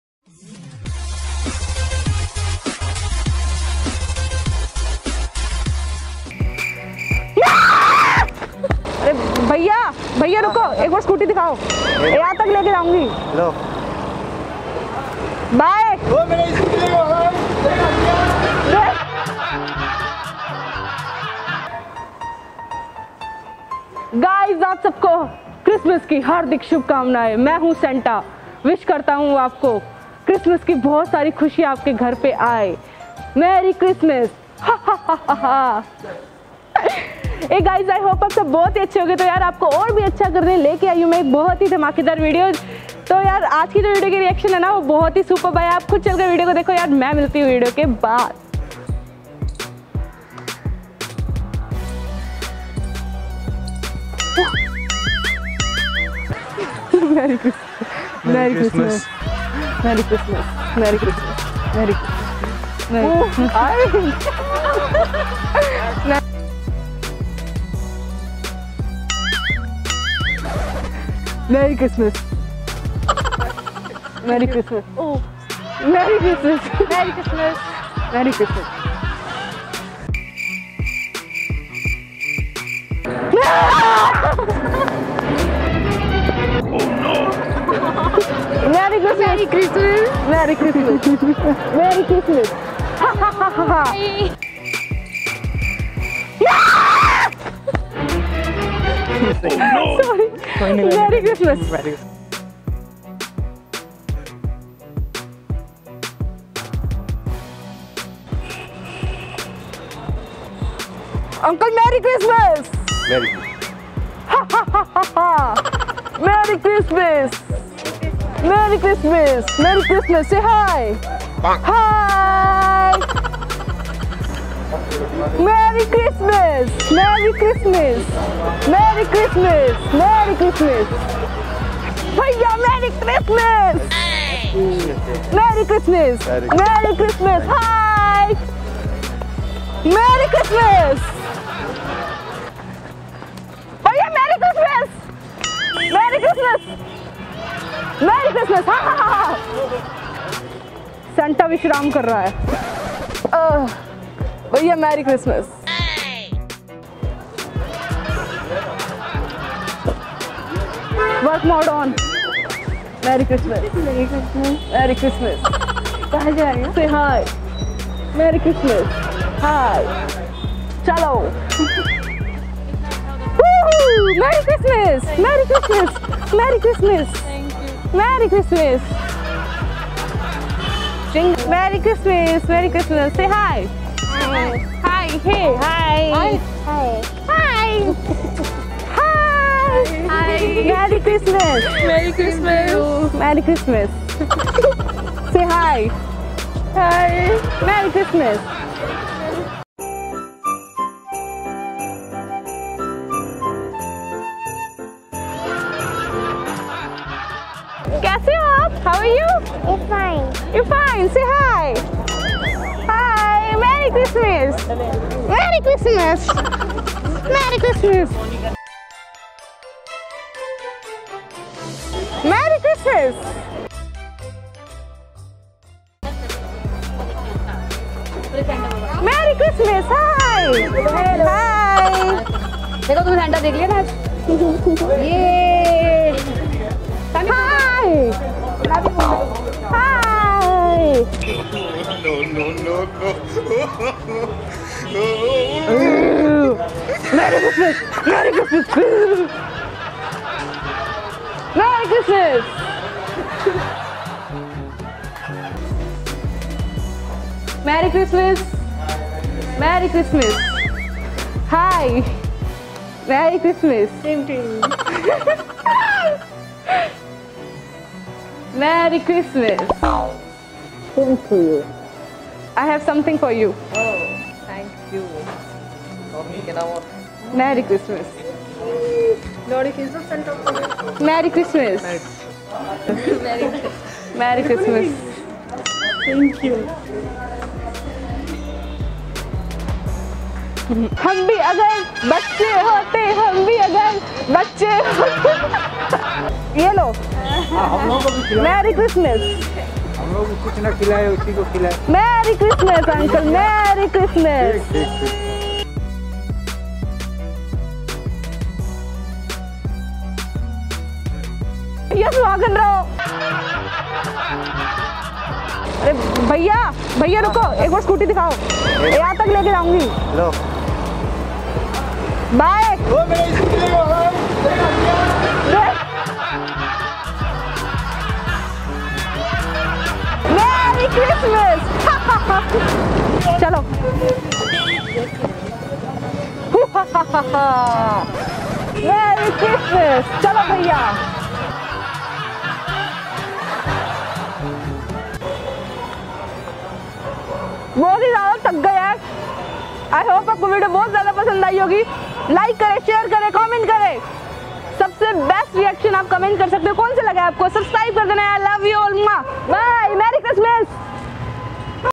Scary Santa Claus Prank | Christmas Special | Rits Dhawan
Prank On Yash Choudhary Part2-

Merry Christmas Family❤